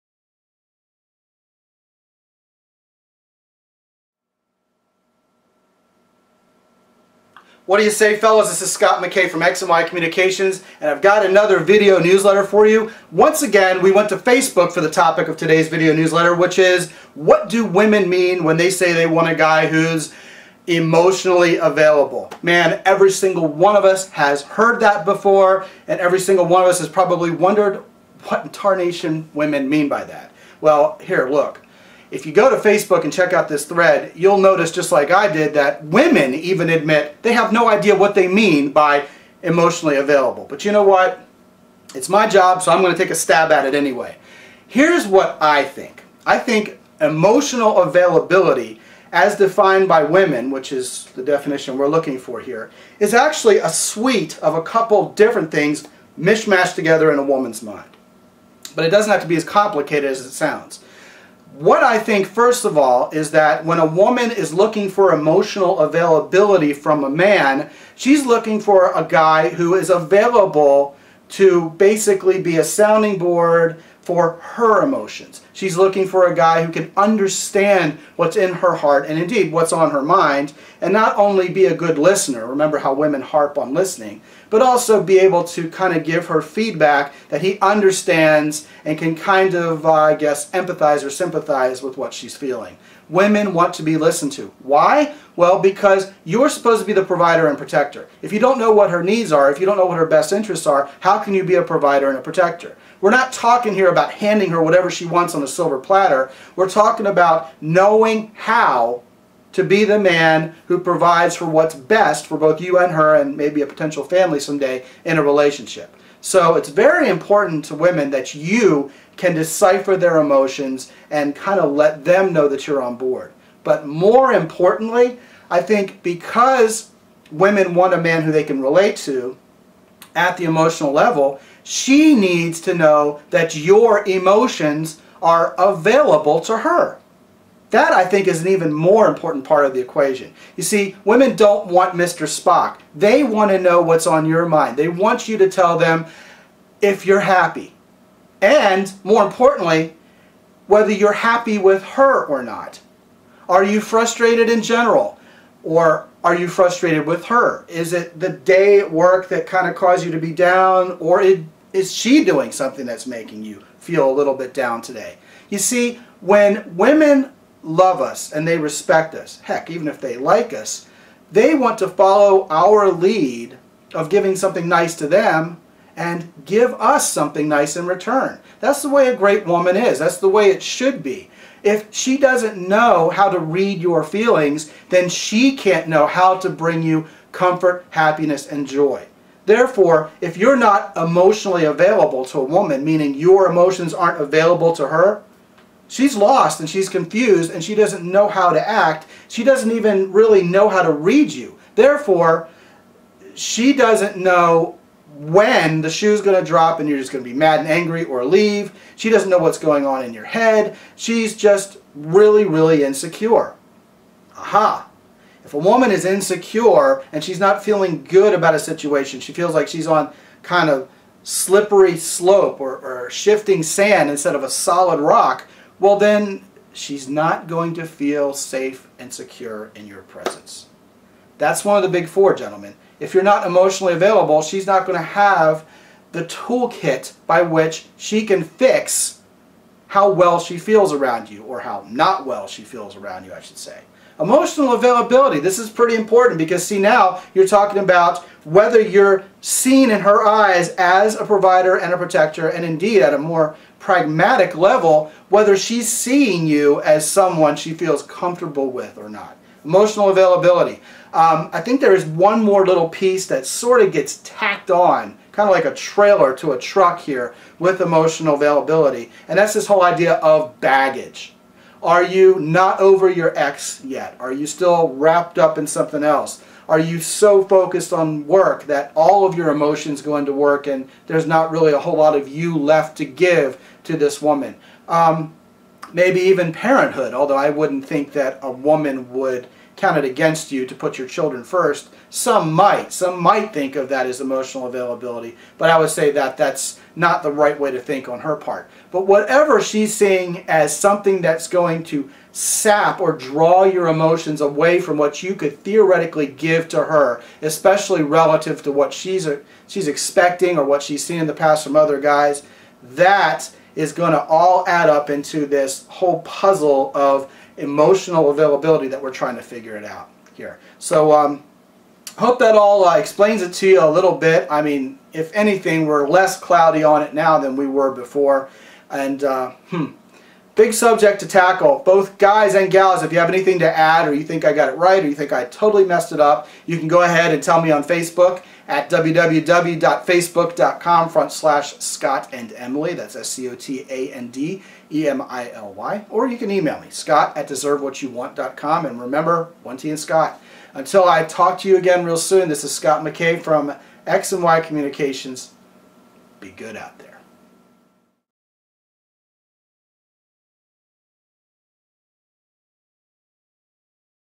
What Does "Emotionally Available" Even Mean?
What is a woman thinking when she says she want's a man who's "emotionally available"?  How do you know if you're that guy...and do you even WANT to be?  All the answers are here in this video.